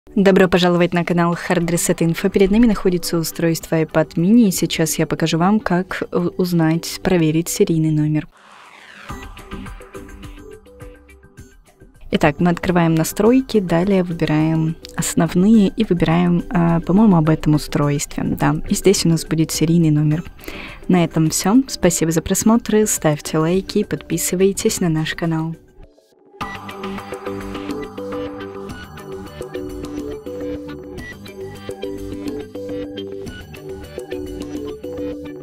APPLE iPad mini | Как проверить серийный номер iPad mini - Где посмотреть серийный номер iPad mini
В этом видео мы покажем, как проверить серийный номер iPad mini. Если вы хотите узнать серийный номер iPad mini, то это видео для вас. Узнайте, как найти серийный номер iPad mini, в этом видео.

Как проверить серийный номер iPad mini? Где посмотреть серийный номер iPad mini? Как узнать серийный номер iPad mini?